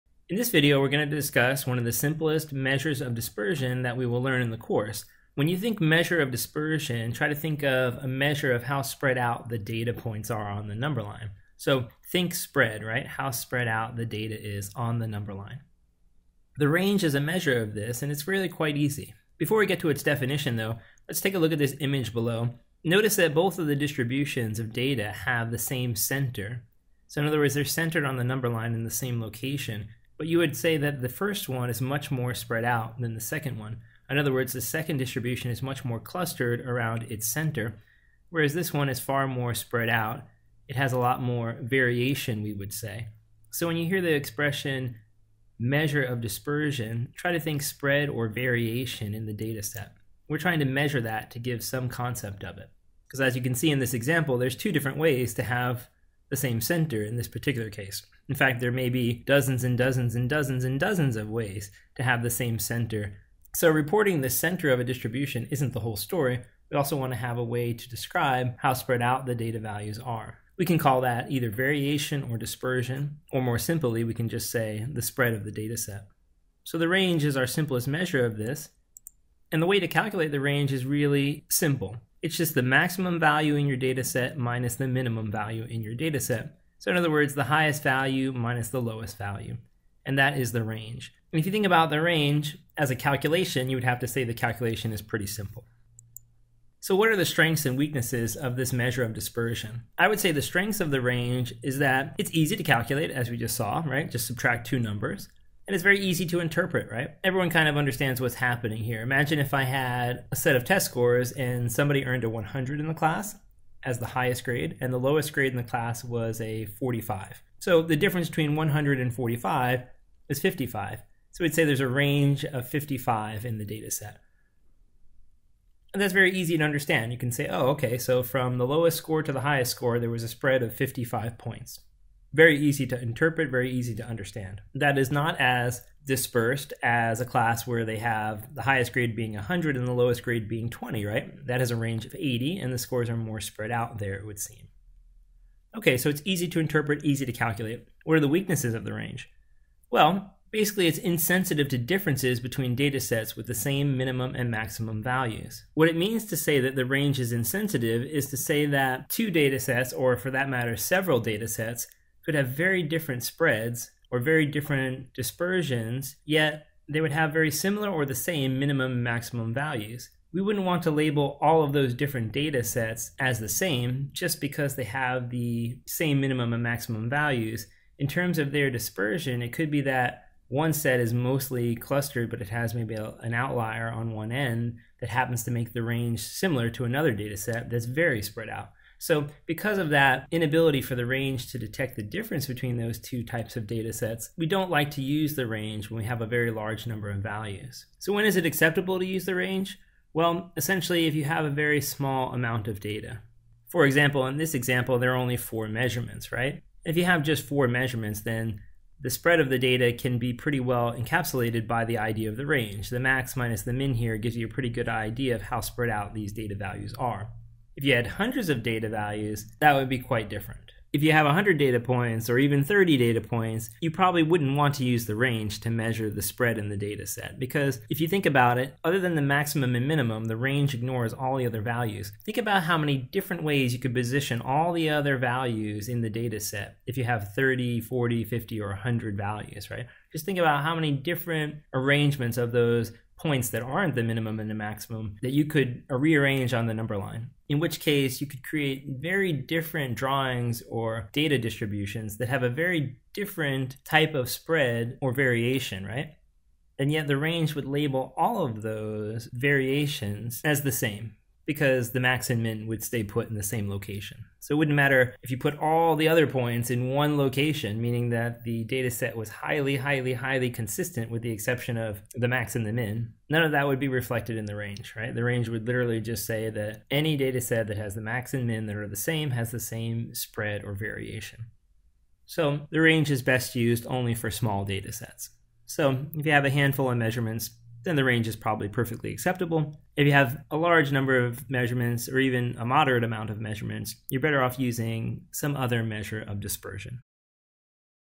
Range
A description of the range as a measure of dispersion.